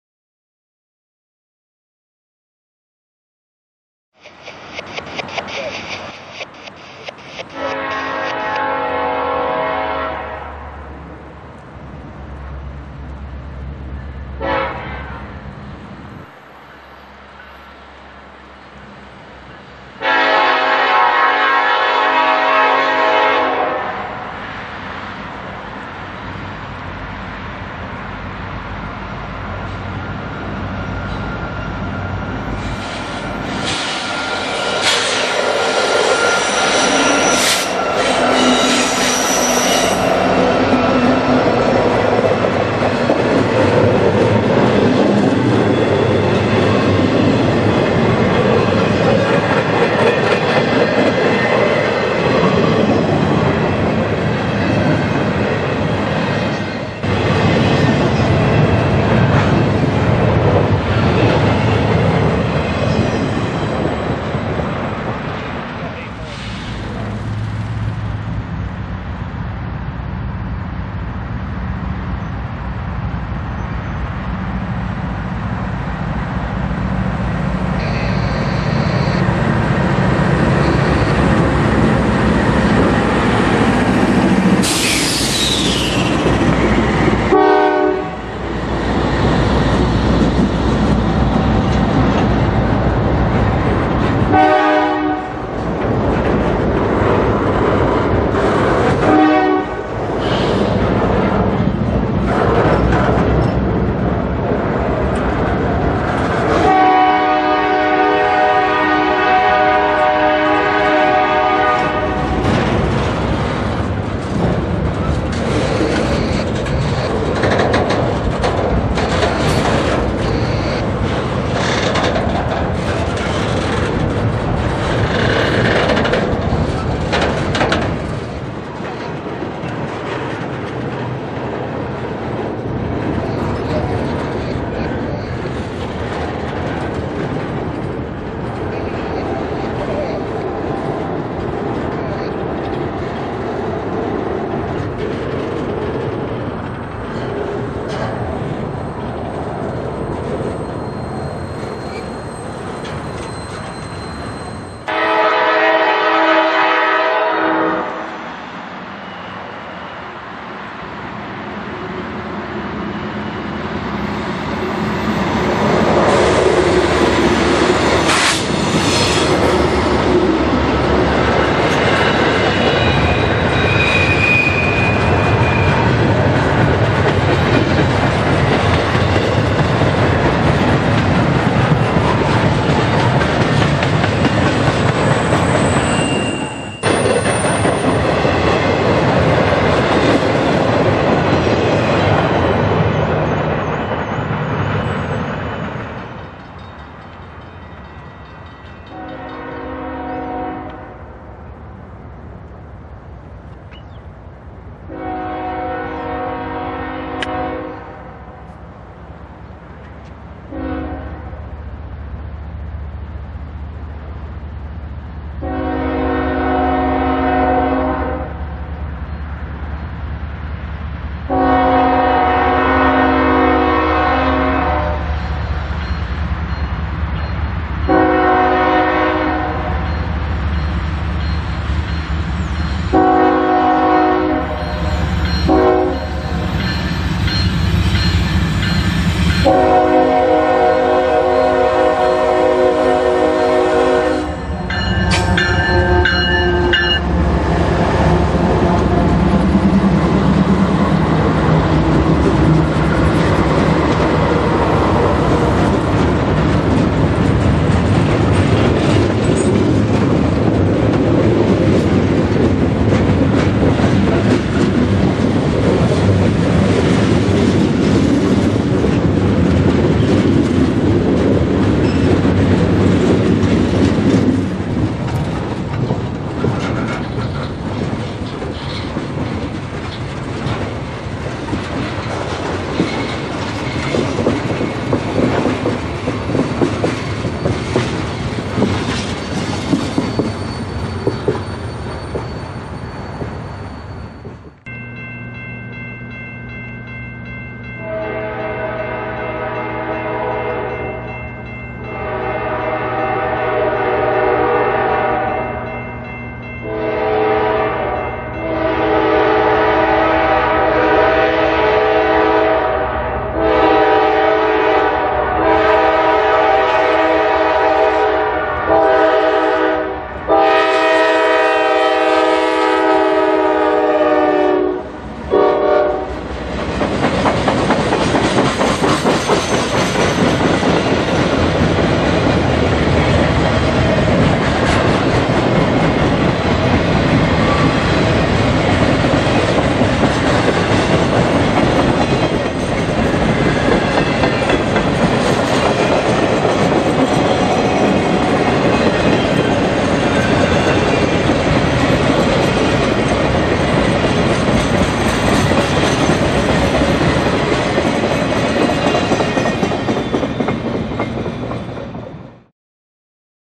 Railfanning in the Atlanta North District 2-21-15
215G119 at Austell GA 135H
Southbound Intermodal Chattanooga TN - Atlanta GA
23NG121 at Austell GA 135H
Northbound Intermodal Atlanta GA - Chattanooga TN
22NG120 at Austell GA 135H
175G119 at Austell GA 135H
Southbound Freight Chattanooga TN - Atlanta GA
251G119 at Austell GA 134H
Southbound Roadrailer Chattanooga TN - Atlanta GA